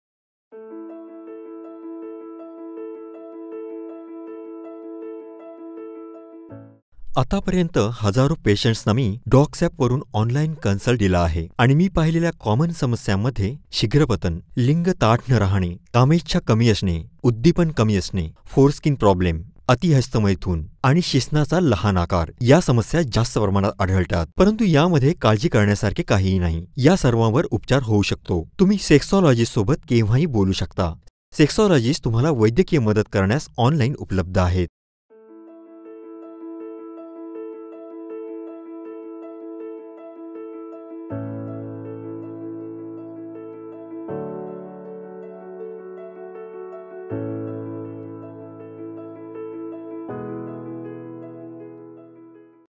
सर्वसामान्यपणे आढळणाऱ्या लैंगिक समस्या कोणत्या आहेत? sex problem konte? AsktheDoctor - DocsAppTv
Dr. Krishan Sharma - MBBS, MD - Psychiatry and Sexology, Sexologist

सर्वसामान्यपणे आढळणाऱ्या लैंगिक समस्या कोणत्या आहेत?

आतापर्यंत हजारो पेशंटना मी DocsApp वरून ऑनलाइन कन्सल्ट दिला आहे,
 आणि मी पाहिलेल्या कॉमन समस्यांमध्ये, शीग्रपतन, लिंग ताठ न राहणे, कामेच्छा कमी असणे, उद्दीपन कमी असणे, फोरस्किन प्रॉब्लेम, अतिहस्तमैथून, आणि शिश्नाचा लहान आकार या समस्या जास्त प्रमाणात आढळतात.  परंतू यामध्ये काळजी करण्यासारखे काहीही नाही, या सर्वांवर उपचार होऊ शकतो.तुम्ही सेक्सॉलॉजिस्ट सोबत केव्हाही बोलू शकता, सेक्सॉलॉजिस्ट तुम्हाला वैद्यकीय मदत करण्यास ऑनलाइन उपलब्ध आहेत.

सर्वसाधारण आढळणाऱ्या समस्या

शीग्रपतन
लिंग ताठ न राहणे
उद्दीपन कमी असणे
कामेच्छा कमी असणे
फोरस्किन प्रॉब्लेम
अतिहस्तमैथून
शिश्नाचा लहान आकार

More Info.

लैंगिक समस्या म्हणेजच सेक्सशुल प्रोबेल्म्स हे खूप कॉमन आहेत, जवळपास प्रत्येकाला आयुष्यात कधीनाकधी लैंगिक समस्या जाणवतात. काही सर्वे मध्ये असे आढळले आहे कि, जवळसपास ३०% स्त्री आणि पुरुषांना काहींना काही लैंगिक समस्या जाणवतात. लैंगिक समस्यांबद्दल बोलणे हे अतिशय कमीपणाचे समजले जाते, त्यामुळे याबद्दल बोलले जात नाही आणि म्हणूनच यावर योग्य उपाय सापडणे कठीण होऊन जाते. पुरुषांमध्ये आढळणाऱ्या सामान्य लैंगिक समस्या या पुढीलप्रमाणे आहेत.

शीग्रपतन, लिंग ताठ न राहणे, कामेच्छा कमी असणे, उद्दीपन कमी असणे,

फोरस्किन प्रॉब्लेम, अतिहस्तमैथून, आणि शिश्नाचा लहान आकार या समस्या जास्त प्रमाणात आढळतात.

स्त्रियांमध्ये आढळणाऱ्या जास्तीत जास्त लैंगिक  समस्या /सेक्स प्रॉब्लेम्स म्हणजे, कामेच्छा कमी असणे, लैगिक समाधान कमी मिळणे, संभोगाच्या वेळी वेदना होणे, पुरेशी उत्तेजना न मिळणे इत्यादी.

या सर्व समस्यांमुळे पती पत्नीमध्ये तणावपूर्वक संबंध निर्माण होऊ शकतात. हे सर्व प्रॉब्लेम्स डॉक्टरांच्या सल्ल्याने निवारण केले जाऊ शकतात. हे डॉक्टर्स ऑनलाइन सुद्धा उपलब्ध आहेत. तुम्ही त्यांच्याशी केव्हाही संपर्क साधू शकता.   स्त्री या स्त्री डॉक्टरांकडून देखील उपचार घेऊ शकतात.